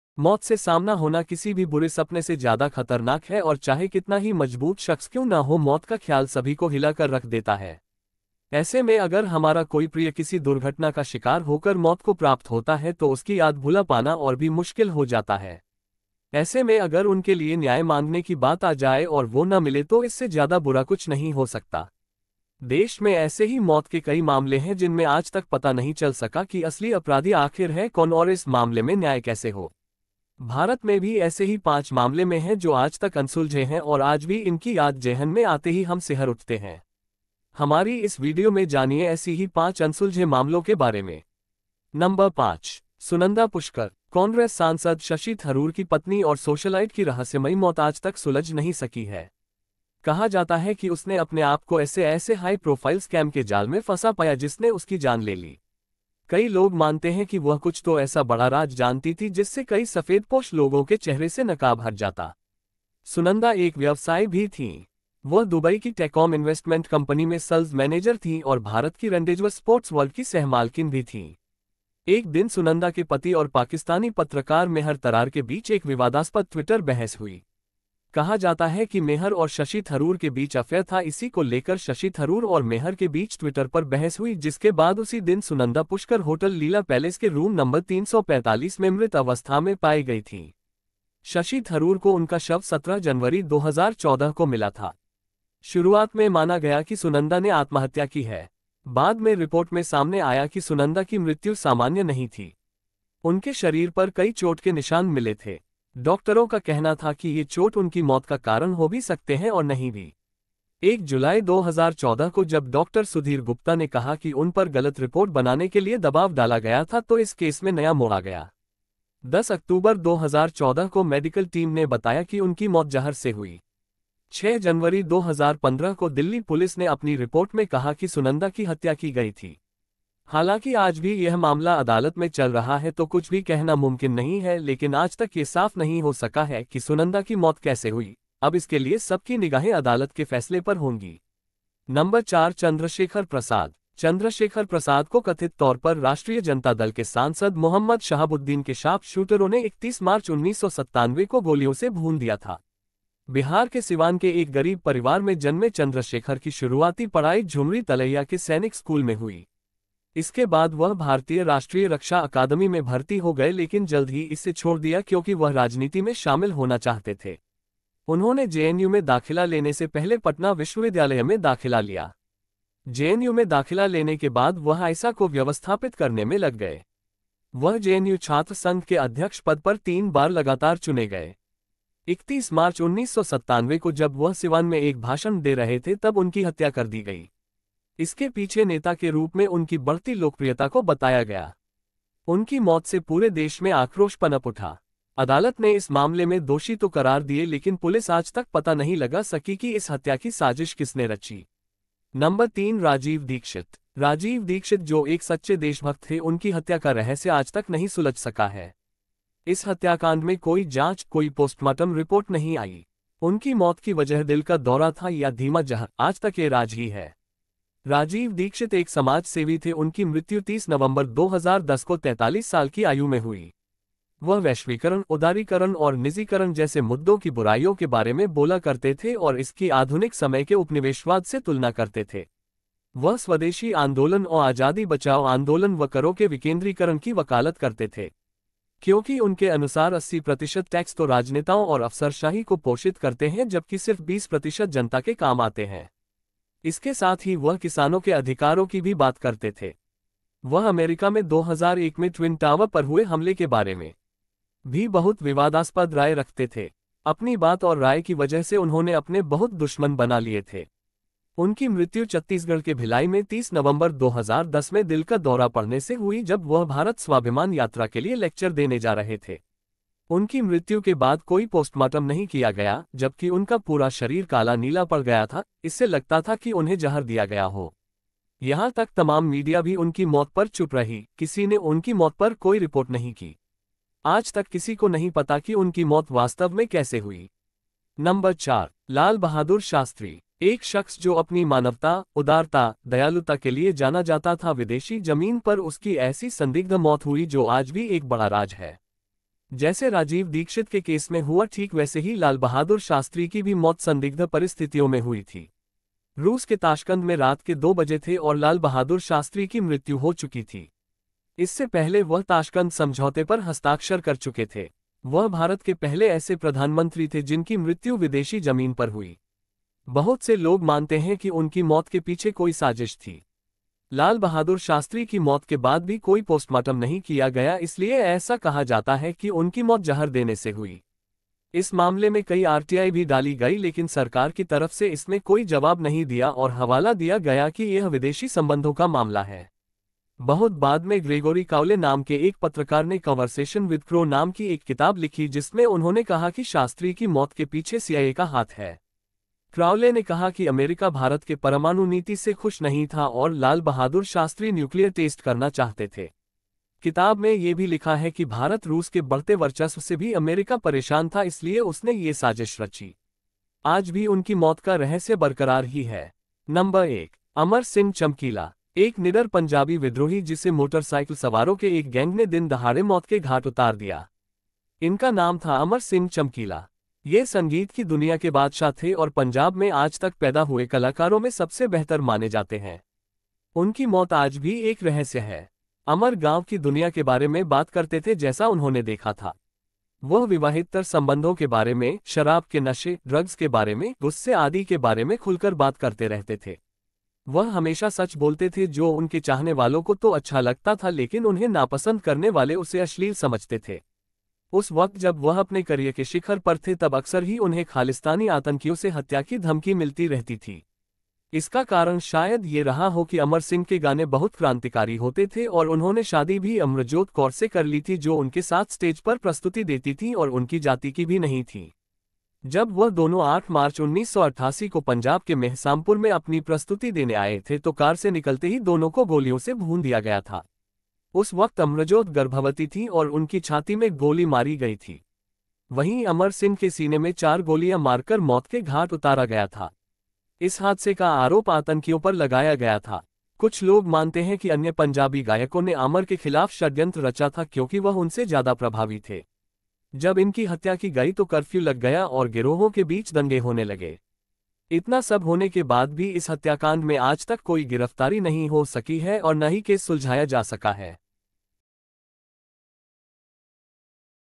भारत के बड़े हत्याकांड जो आज भी हैं अनसुलझे। Big murder cases of India which are still unsolved 😱।।
इस वीडियो में हम जानेंगे , भारत के वो 5 बड़े हत्याकांड जो आज भी हैं अनसुलझे, हर केस आपको हैरान कर देगा

Those 5 big murder cases of India which are still unsolved, each case will surprise you ..

मौत से सामना होना किसी भी बुरे सपने से ज्यादा खतरनाक है और चाहे कितना ही मजबूत शख्स क्यों न हो मौत का ख्याल सभी को हिला कर रख देता है। ऐसे में अगर हमारा कोई प्रिय किसी दुर्घटना का शिकार होकर मौत को प्राप्त होता है तो उसकी याद भुला पाना और भी मुश्किल हो जाता है। ऐसे में अगर उनके लिए न्याय मांगने की बात आ जाए और वो न मिले तो इससे ज्यादा बुरा कुछ नहीं हो सकता।

देश में ऐसे ही मौत के कई मामले हैं जिनमें आज तक पता नहीं चल सका कि असली अपराधी आखिर है कौन और इस मामले में न्याय कैसे हो। भारत में भी ऐसे ही 5 मामले में हैं जो आज तक अनसुलझे हैं और आज भी इनकी याद जेहन में आते ही हम सिहर उठते हैं। हमारी इस स्टोरी में पढ़िए ऐसी ही 5 अनसुलझे मामलों के बारे में...

5. सुनंदा पुष्कर

कांग्रेस सांसद शशि थरूर की पत्नी और सोशलाइट की रहस्यमयी मौत आज तक सुलझ नहीं सकी है। कहा जाता है कि उसने अपने आप को ऐसे-ऐसे हाई प्रोफाइल स्कैम के जाल में फंसा पाया जिसने उसकी जान ले ली। कई लोग मानते हैं कि वह कुछ तो ऐसा बड़ा राज जानती थी जिससे कई सफेदपोश लोगों के चेहरे से नकाब हट जाता।

सुनंदा एक व्यवसायी भी थीं। वह दुबई की टेकॉम इंवेस्टमेंट कंपनी में सेल्स-मैनेजर थीं और भारत की रेंडेजवस स्पोर्ट्स वर्ल्ड की सह-मालकिन भी थीं। एक दिन सुनंदा के पति और पाकिस्तानी पत्रकार मेहर तरार के बीच एक विवादास्पद ट्विटर बहस हुई। कहा जाता है कि मेहर और शशि थरूर के बीच अफेयर था इसी को लेकर शशि थरूर और मेहर के बीच ट्विटर पर बहस हुई जिसके बाद उसी दिन सुनंदा पुष्कर होटल लीला पैलेस के रूम नंबर 345 में मृत अवस्था में पाई गई थीं। शशि थरूर को उनका शव 17 जनवरी 2014 को मिला था।

शुरुआत में माना गया कि सुनंदा ने आत्महत्या की है। बाद में रिपोर्ट में सामने आया कि सुनंदा की मृत्यु सामान्य नहीं थी। उनके शरीर पर कई चोट के निशान मिले थे। डॉक्टरों का कहना था कि ये चोट उनकी मौत का कारण हो भी सकते हैं और नहीं भी। 1 जुलाई 2014 को जब डॉक्टर सुधीर गुप्ता ने कहा कि उन पर गलत रिपोर्ट बनाने के लिए दबाव डाला गया था तो इस केस में नया मोड़ आ गया।

10 अक्तूबर 2014 को मेडिकल टीम ने बताया कि उनकी मौत जहर से हुई। 6 जनवरी 2015 को दिल्ली पुलिस ने अपनी रिपोर्ट में कहा कि सुनंदा की हत्या की गई थी। हालांकि आज भी यह मामला अदालत में चल रहा है तो कुछ भी कहना मुमकिन नहीं है लेकिन आज तक यह साफ नहीं हो सका है कि सुनंदा की मौत कैसे हुई, अब इसके लिए सबकी निगाहें अदालत के फैसले पर होंगी।

चंद्रशेखर प्रसाद

चंद्रशेखर प्रसाद को कथित तौर पर राष्ट्रीय जनता दल के सांसद मोहम्मद शहाबुद्दीन के शार्प शूटरों ने 31 मार्च 1997 को गोलियों से भून दिया था। बिहार के सिवान के एक गरीब परिवार में जन्मे चंद्रशेखर की शुरुआती पढ़ाई झुमरी तलैया के सैनिक स्कूल में हुई। इसके बाद वह भारतीय राष्ट्रीय रक्षा अकादमी में भर्ती हो गए लेकिन जल्द ही इसे छोड़ दिया क्योंकि वह राजनीति में शामिल होना चाहते थे। उन्होंने जेएनयू में दाखिला लेने से पहले पटना विश्वविद्यालय में दाखिला लिया।

जेएनयू में दाखिला लेने के बाद वह आईसा को व्यवस्थापित करने में लग गए। वह जेएनयू छात्र संघ के अध्यक्ष पद पर तीन बार लगातार चुने गए। 31 मार्च 1997 को जब वह सिवान में एक भाषण दे रहे थे तब उनकी हत्या कर दी गई। इसके पीछे नेता के रूप में उनकी बढ़ती लोकप्रियता को बताया गया। उनकी मौत से पूरे देश में आक्रोश पनप उठा। अदालत ने इस मामले में दोषी तो करार दिए लेकिन पुलिस आज तक पता नहीं लगा सकी कि इस हत्या की साजिश किसने रची।

3 राजीव दीक्षित

राजीव दीक्षित जो एक सच्चे देशभक्त थे उनकी हत्या का रहस्य आज तक नहीं सुलझ सका है। इस हत्याकांड में कोई जांच, कोई पोस्टमार्टम रिपोर्ट नहीं आई। उनकी मौत की वजह दिल का दौरा था या धीमा जहर, आज तक यह राज ही है।

राजीव दीक्षित एक समाज सेवी थे उनकी मृत्यु 30 नवंबर 2010 को 43 साल की आयु में हुई। वह वैश्वीकरण, उदारीकरण और निजीकरण जैसे मुद्दों की बुराइयों के बारे में बोला करते थे और इसकी आधुनिक समय के उपनिवेशवाद से तुलना करते थे। वह स्वदेशी आंदोलन औ आजादी बचाओ आंदोलन व करों के विकेंद्रीकरण की वकालत करते थे। क्योंकि उनके अनुसार 80 प्रतिशत टैक्स तो राजनेताओं और अफसरशाही को पोषित करते हैं जबकि सिर्फ 20 प्रतिशत जनता के काम आते हैं। इसके साथ ही वह किसानों के अधिकारों की भी बात करते थे।

वह अमेरिका में 2001 में ट्विन टावर पर हुए हमले के बारे में भी बहुत विवादास्पद राय रखते थे। अपनी बात और राय की वजह से उन्होंने अपने बहुत दुश्मन बना लिए थे। उनकी मृत्यु छत्तीसगढ़ के भिलाई में 30 नवंबर 2010 में दिल का दौरा पड़ने से हुई जब वह भारत स्वाभिमान यात्रा के लिए लेक्चर देने जा रहे थे। उनकी मृत्यु के बाद कोई पोस्टमार्टम नहीं किया गया, जबकि उनका पूरा शरीर काला-नीला पड़ गया था, इससे लगता था कि उन्हें जहर दिया गया हो। यहां तक तमाम मीडिया भी उनकी मौत पर चुप रही, किसी ने उनकी मौत पर कोई रिपोर्ट नहीं की। आज तक किसी को नहीं पता कि उनकी मौत वास्तव में कैसे हुई।.